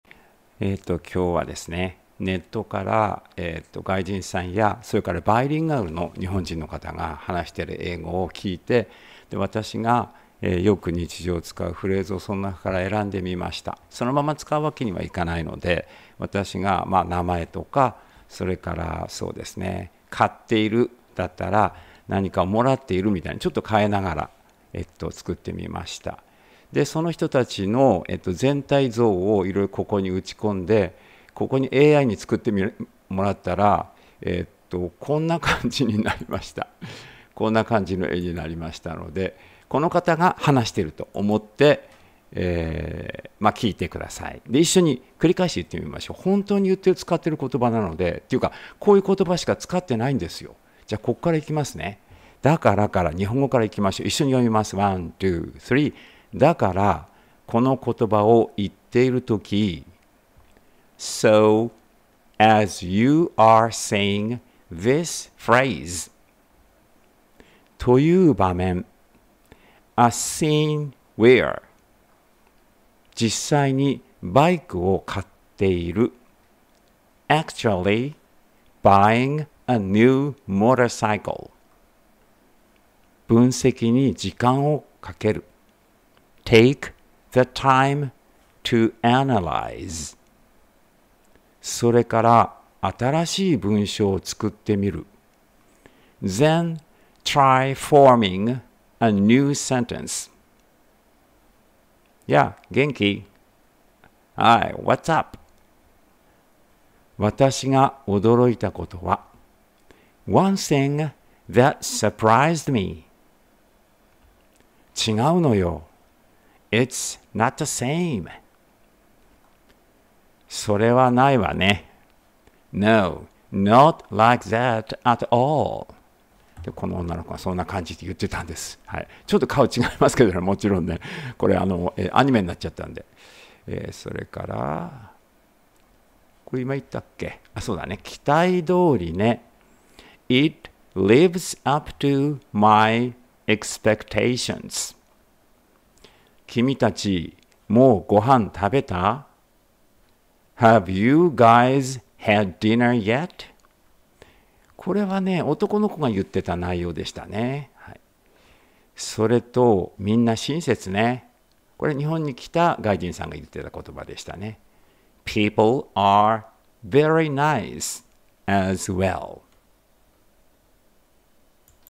えいご おんどく 27
だからこのことばをいっているとき　so as you are saying this phrase

というばめん　a scene where

実際にバイクを買っている　actually buying a new motorcycle

分析にじかんをかける　take the time to analyze

それからあたらしい文章をつくってみる　then try forming a new sentence

やあ、げんき？　Hi, what’s up?

わたしがおどろいたことは　one thing that surprised me

ちがうのよ。　It’s not the same.

それはないわね。　No, not like that at all.

期待どおりね。　It lives up to my expectations.

きみたち、もうごはん食べた？　Have you guys had diner yet?

それと、みんな親切ね。　People are very nice as well.